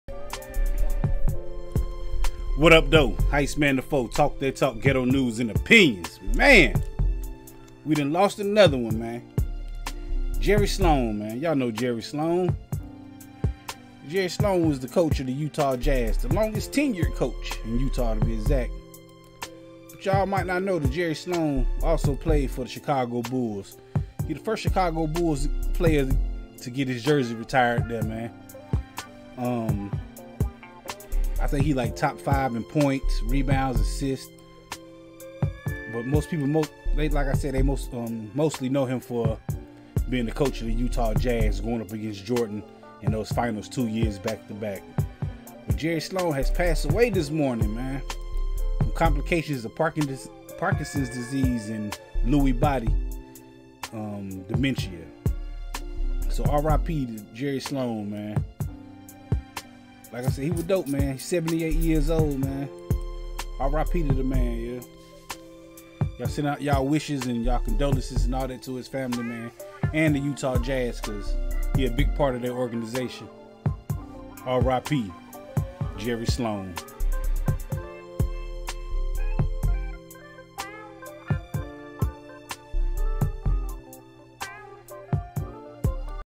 Jerry Sloan DEAD at age 78 R.I.P
Long time Utah Jazz Coach Jerry Sloan has passed away this morning due to complications from Parkinson's Disease and Lewy body dementia.